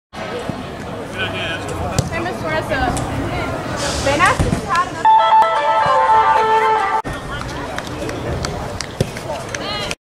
9.17.2021 "Thank You" McGill-Toolen for hosting the St. Michael vs Escambia County Football Game.
The St. Michael community would like to Thank everyone at McGill-Toolen for hosting the St. Michael vs Escambia County football game Thursday night, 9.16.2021.
With all the weather and rain St. Michael was not able to play at W.C. Majors field at the last minute. We would like to Thank everyone at McGill who came out to help, Administrators, Staff, Coaches & Concessions.
It was a great night for St. Michael Cardinal football and we thank
McGill-Toolen for everything they did to prepare the Archbishop Oscar H. Lipscomb Complex - "The Lip" Stadium for our Thursday night football game.

St. Michael Catholic High School. Fairhope, Alabama
St. Michael, Defend Us!